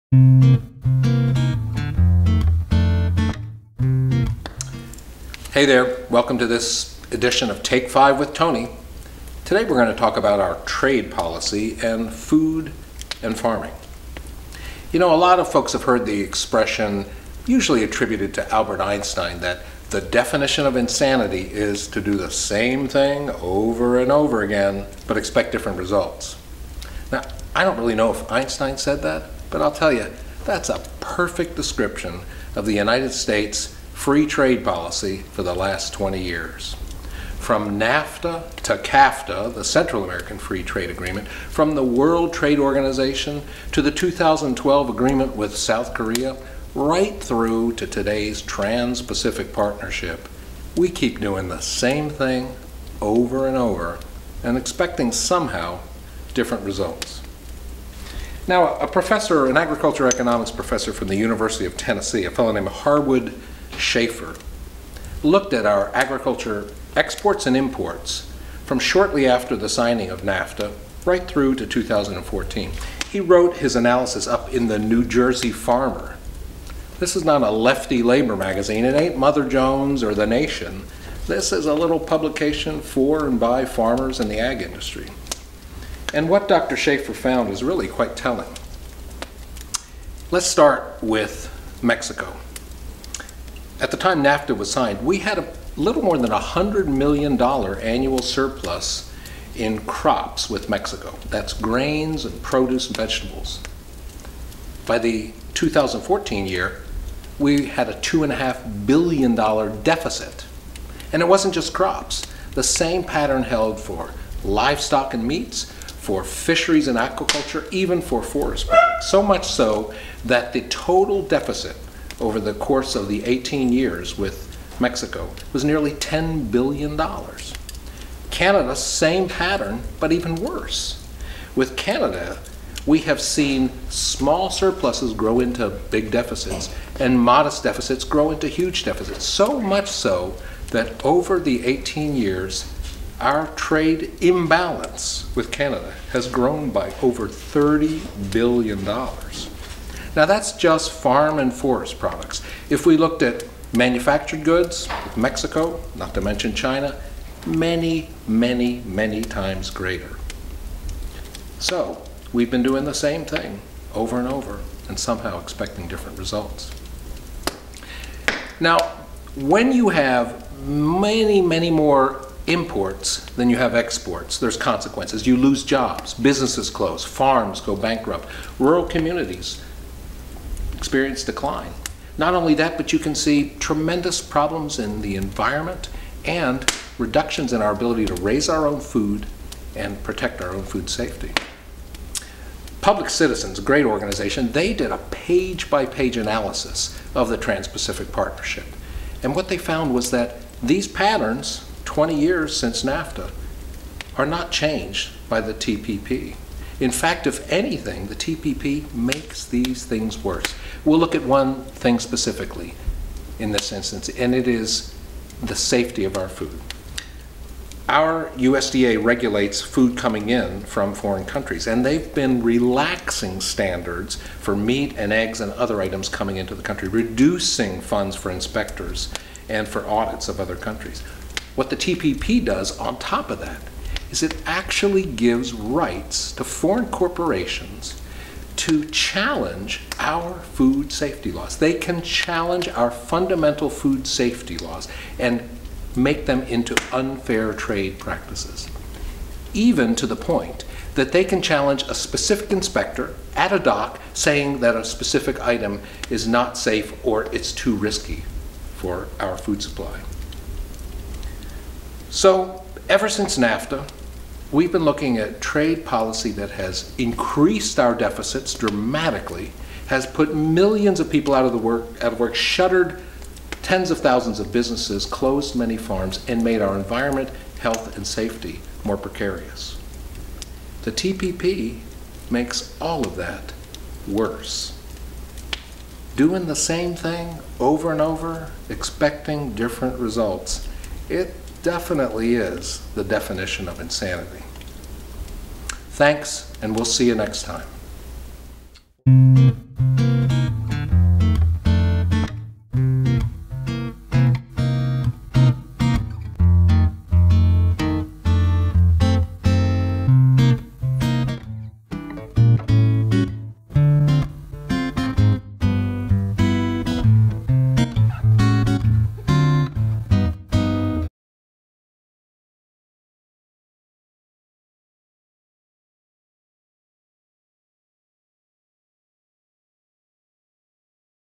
Trade Policy - Take 5 with Tony (S.3, Ep.2)
In the second episode of season 3, Anthony gives a fireside chat about US trade.